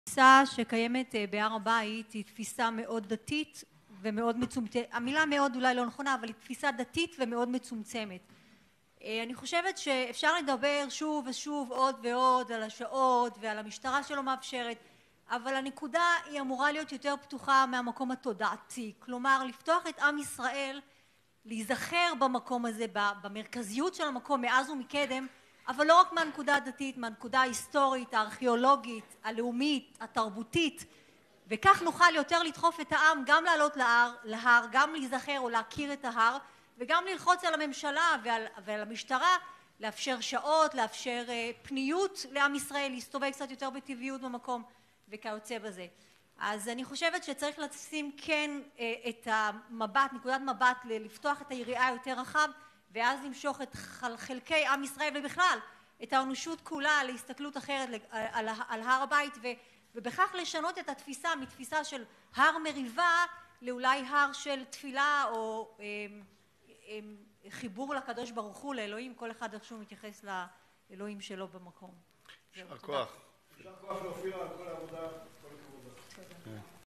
כנס הר הבית בכנסת, דברי אופירה, שיחות בהר הבית, בנושא עידוד עליה להר
בכנס שנערך אתמול בכנסת בנושא עידוד יהודים לעלות להר הבית, גם אני הייתי נוכחת בקהל והשמעתי את תפיסתי באשר להר לעידוד יהודים ובכלל לעלות להר הבית וכן איך אפשר לשנות את התפיסה הרווחת מהר מריבה להר של תפילה ופולחן לכל העמים. ובכך להביא את השלום על המקום! ובא לציון גואל